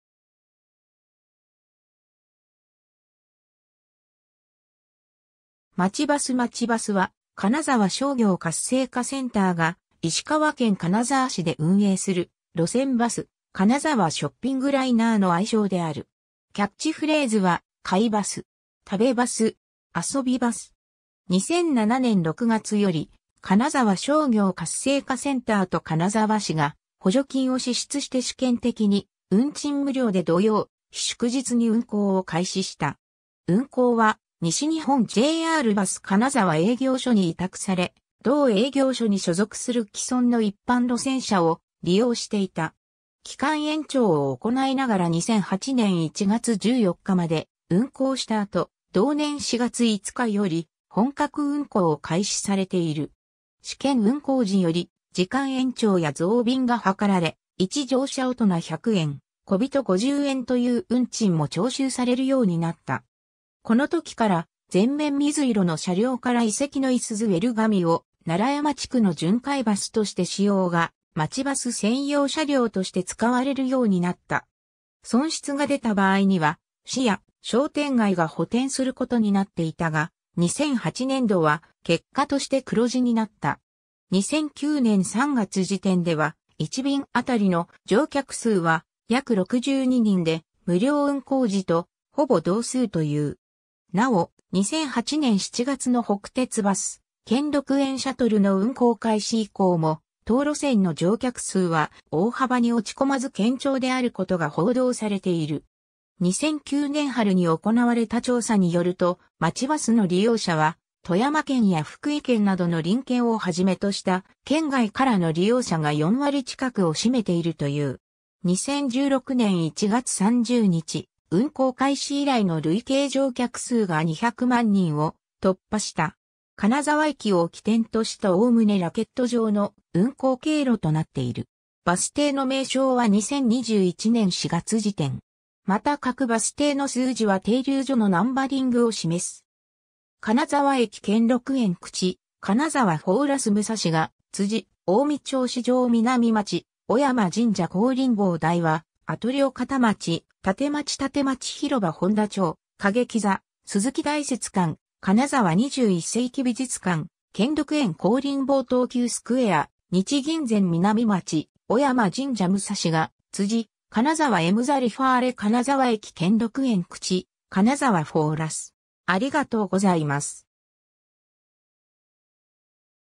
まちバス
まちバス（金沢駅東口にて） まちバスは、金沢商業活性化センターが、石川県金沢市で運営（運行委託先は西日本ジェイアールバス）する路線バス「金沢ショッピングライナー」の愛称である。
キャッチフレーズは『買いバス。
食べバス。
遊びバス。
』 2007年6月より金沢商業活性化センター（金沢市出資の第3セクター）と金沢市が補助金を支出して試験的に運賃無料で土曜･日祝日に運行を開始した。
運行は西日本JRバス金沢営業所に委託され、同営業所に所属する既存の一般路線車を利用していた。
 期間延長を行いながら2008年1月14日まで運行した後、同年4月5日より本格運行を開始（当初は2009年3月29日までの運行予定、のち延長）されている。
試験運行時より時間延長や増便が図られ、1乗車大人100円、小人50円という運賃も徴収されるようになった。
このときから全面水色の車両（西日本JRバス平城山派出所（2008年3月廃止）から移籍のいすゞ・エルガミオ、平城山地区の巡回バスとして使用）が「まちバス」専用車両として使われるようになった。
 損失が出た場合には市や商店街が補填することになっていたが、2008年度は結果として黒字になった。
2009年3月時点では、一便あたりの乗客数は約62人で、無料運行時とほぼ同数という。
 なお、2008年7月の北鉄バス「兼六園シャトル」（2021年3月で廃止）の運行開始以降も、当路線の乗客数は、大幅に落ち込まず堅調であることが報道されている。
 2009年春に行われた調査によると、まちバスの利用者は、富山県や福井県などの隣県を始めとした「県外」からの利用者が4割近くを占めているという。
 2016年1月30日、運行開始以来の累計乗客数が200万人を突破した。
 金沢駅を起点とした概ねラケット状の運行経路となっている。
バス停の名称は2021年4月時点。
また各バス停の数字は停留所のナンバリングを示す。
 金沢駅兼六園口（東口）・金沢フォーラス 武蔵ヶ辻・近江町市場 南町・尾山神社 香林坊大和・アトリオ 片町・タテマチ タテマチ広場 本多町・歌劇座・鈴木大拙館 金沢21世紀美術館・兼六園（真弓坂口） 香林坊東急スクエア・日銀前 南町・尾山神社 武蔵ヶ辻・金沢エムザ リファーレ 金沢駅兼六園口（東口）・金沢フォーラス